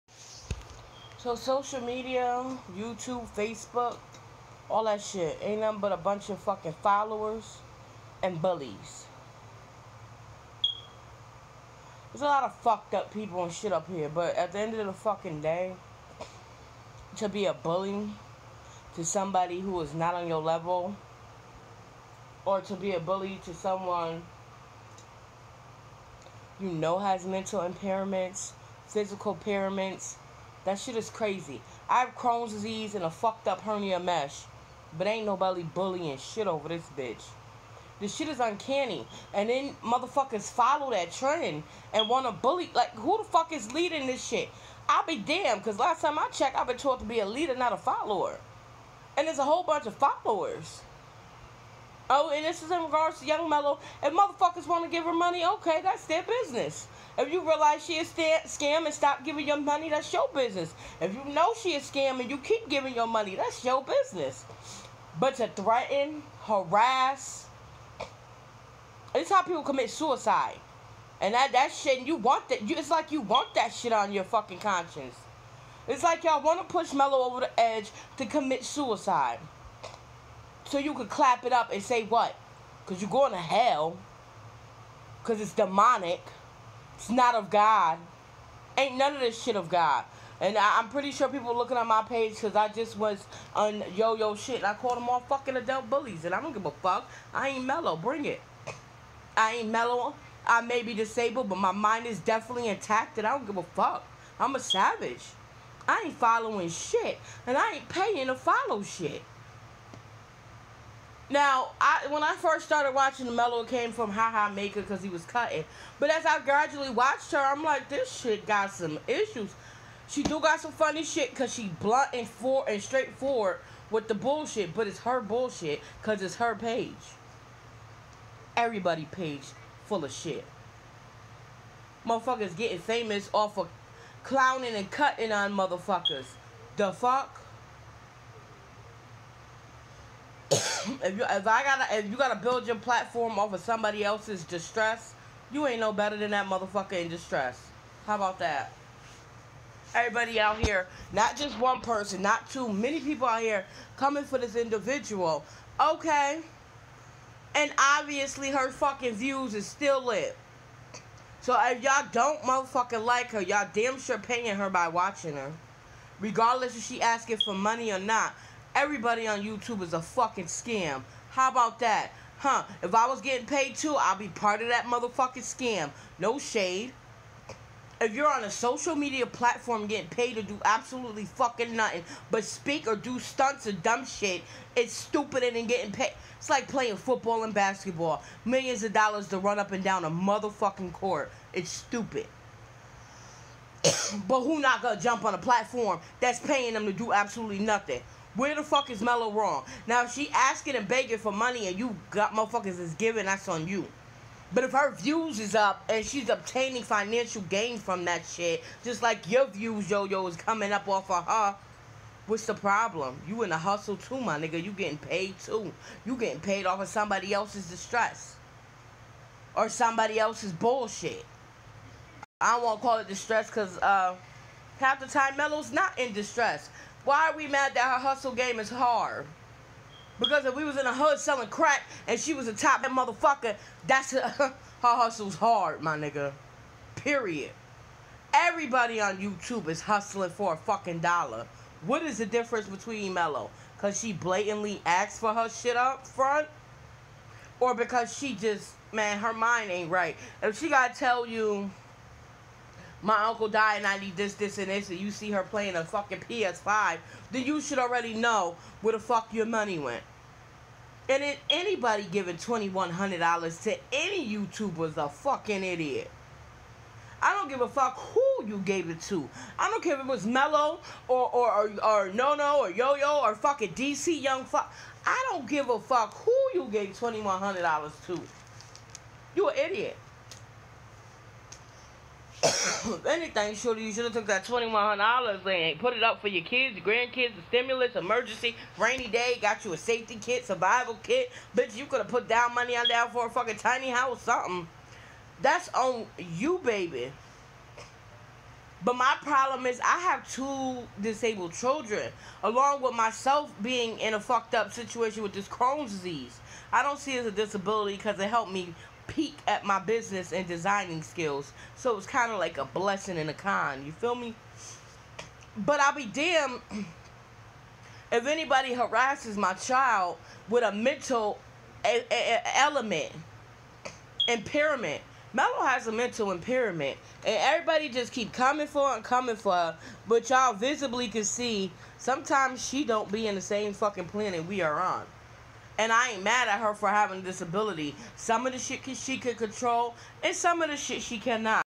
I got blocked for defending melo who is being bullied by adults.. she may he wrong, but.....
2020 is sad when u cant voice a different opinion without getting blocked n have a logical conversation. maybe im missing something..maybe you are ...its where u listen n learn ..this new era🙃🙃🙃 if u dont agree u aint on my team..u right
nigga im a leader i follow no one... especially flawed flesh ... GTFO...but sad cant have an adult convo.. means 2020 dont have many adults. just followers LMAO.

Weather she wrong or right everyone getting paid off youtube a scammer than...if i got paid id feel like i have to offer something worth being paid for. not commentary...

Her mentality isn't up to par with a lot of adults harassing her... But when u threaten someone's life over social media. n her hustle... YouTube aint shit, but a mf hustle. i dont follow a gang of followers.. n get rich off it. I mean I seen like 12&15 in my conclusion.
mf getting $$ off dissing melo.. so they scammers to... If she wrong report it, but to get a gang of followers. or threatening her .. that's sadistic .af...
. Glad im a leader..

Pssst..
my fat beautiful ass isn't melo... Dont try me... My street name pimpstrezz... N own my own company i don't need youtube to get right or viral ... especially off others flawes
..like we dont all have shit mf dont like n shit we do that's not right..

she may be wrong, but so are u mf ..